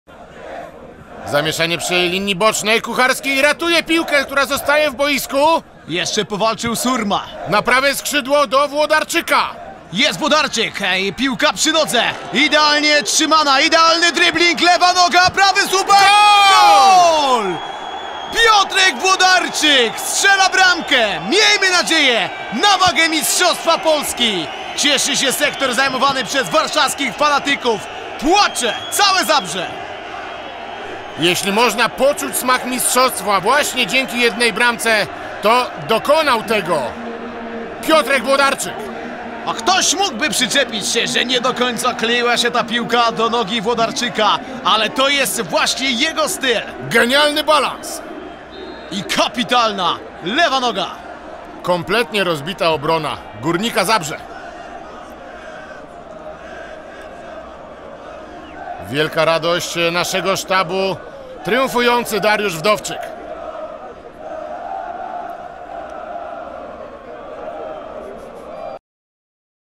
Powtórka z rozgrywki: Górnik Zabrze - Legia Warszawa, Gol: Piotr Włodarczyk
W cyklu ''Powtórka z rozgrywki'' prezentujemy najpiękniejsze bramki zdobyte przez Legię w ciągu ostatnich kilku lat. Dzisiaj przypominamy gola Piotra Włodarczyka, strzelonego Górnikowi Zabrze dającego Legii Mistrzostwo Polski w sezonie 2005/2006!Polub nasz kanał i bądź z nami na bieżąco!